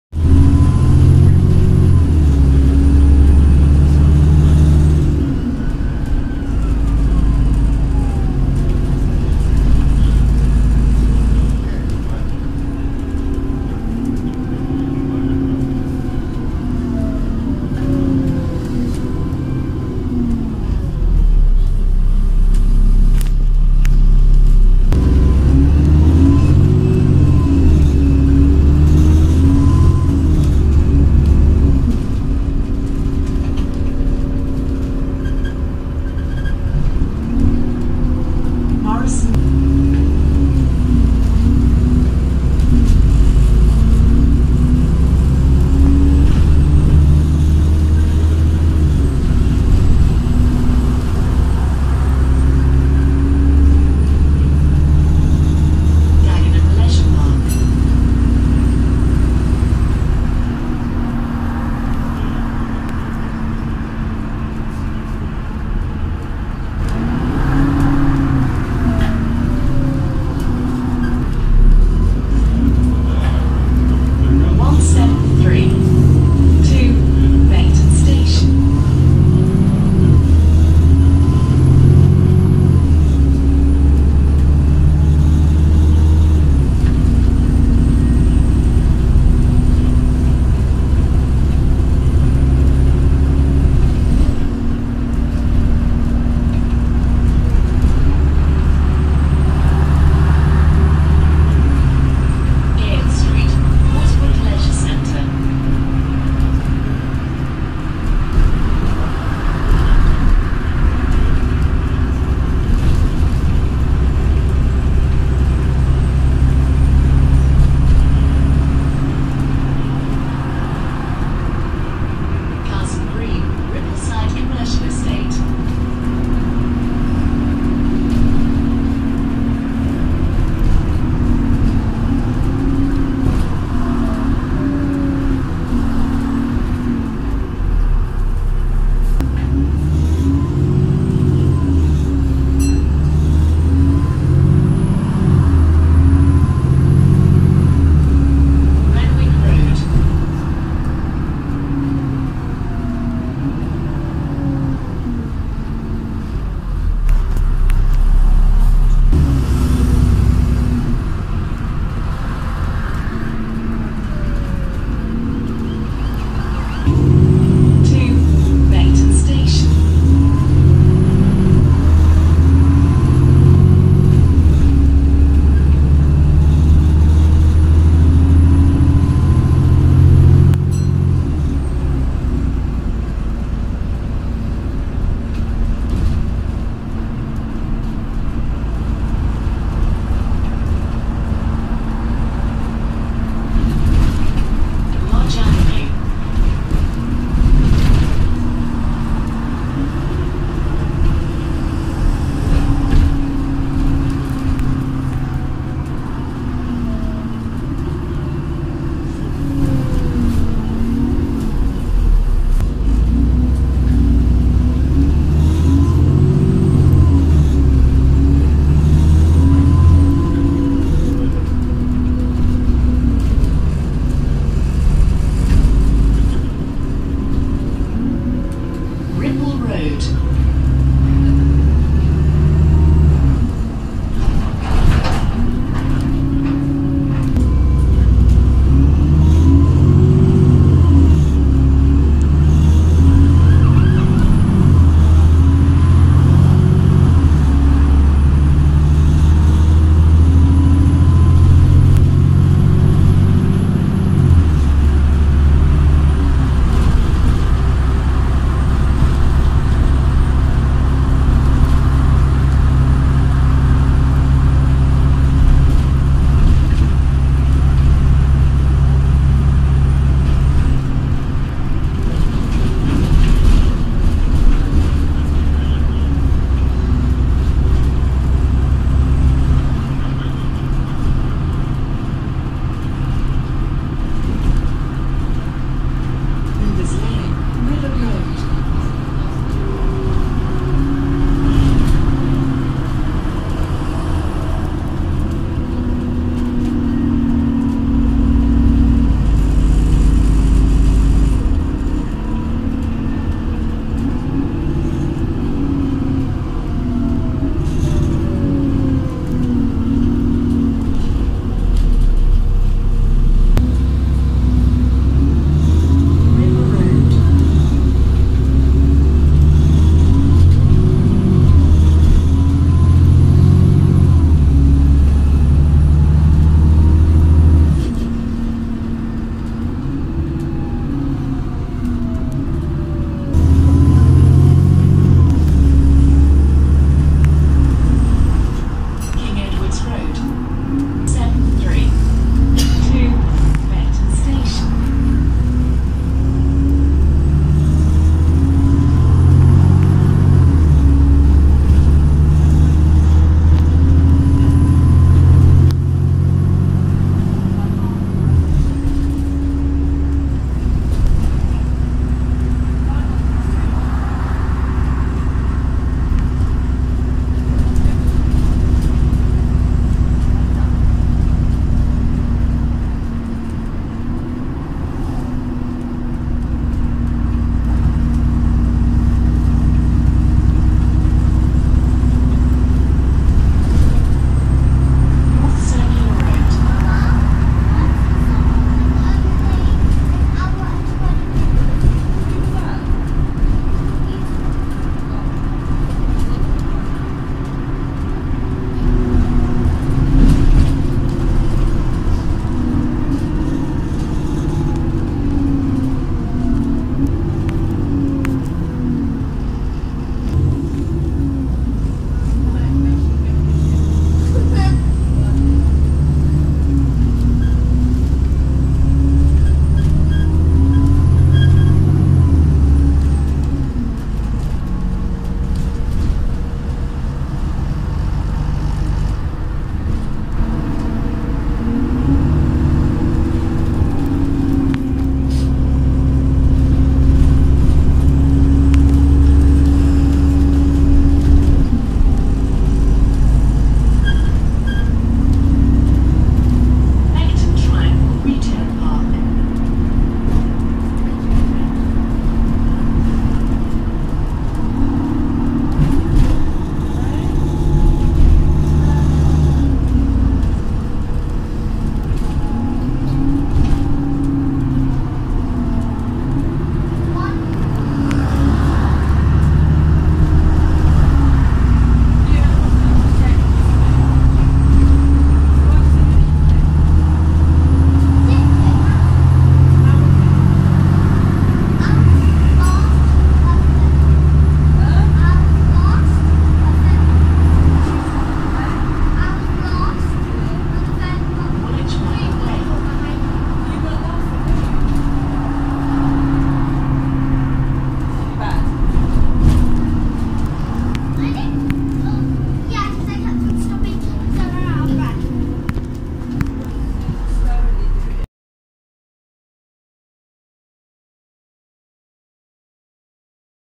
(THRASH) + (TOPODYN BEAST) arriva London 173 to Beckton station T193 LJ60 ATX
Some nice thrash on T193 which is in the process of being refurbished as if you can tell the difference between the seat colourings half the seats in the bus are new (blue) whilst the other half of seats are worn out and greenish grey colour and haven't been cleaned since 2010

Last 173 video for now I promise but there will be more in the next few weeks

Vehicle information
Bus type E400
Chassis ADL E40D trident??
Emissions euro 5 (euro 6 convert)
Gearbox 6 speed ZF ecolife topodyn enabled
New in 2010 (9 years old)
Built (presumably ADL Falkirk but registered in London)
Condition still in service

New for route 150 but refurbishments on this batch only started November 2018 I believe with T180 being the first

The 173 is operated by arriva London at Barking DX

© Daniel's UK Transport videography 2019